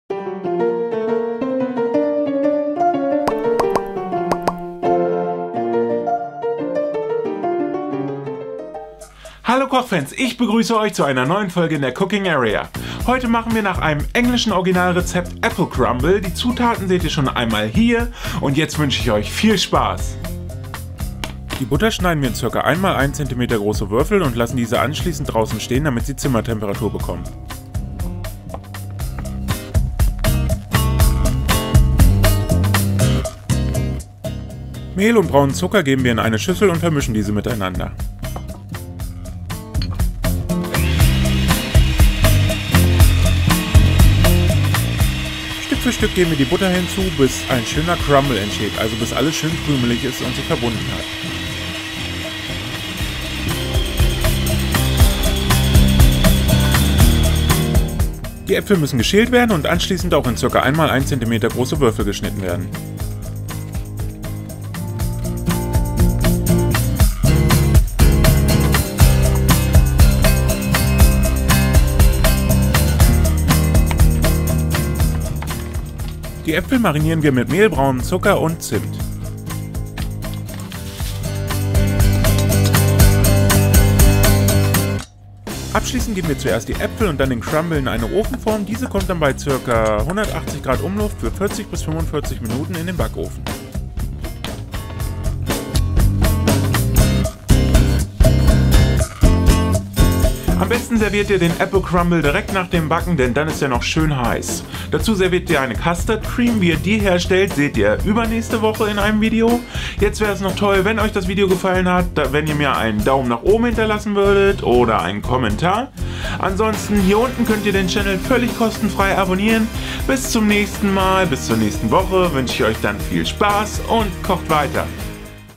APPLE CRUMBLE | Schnell & Einfach Selber kochen | CookingAREA | Rezepte und Tipps 2015
Mein neuer Film Apple Crumble | Schnell & Einfach Selber kochen | CookingAREA | Rezepte und Tipps 2015

Zutaten
Für den Crumble

300g Vollkornmehl
180g brauner Zucker
200g Butter

Für die Äpfel

500g Apfel (Boskop)
30g brauner Zucker
1 EL Vollkornmehl
1 TL Zimt